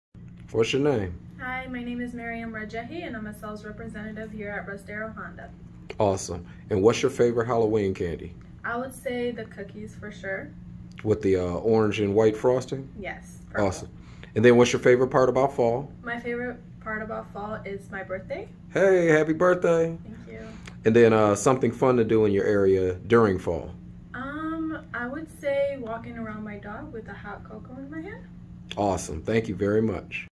Russ Darrow Honda Fall Staff Highlight
Russ Darrow Staff Highlights at Russ Darrow Honda, Fall Edition! We asked our staff the following questions, check the video out to see their answer.

1. Favorite Halloween Candy
2. Favorite part about fall
3. Something fun to do in your area during fall

Stop in today and let's get you in a new vehicle to enjoy your fall activities in 🚗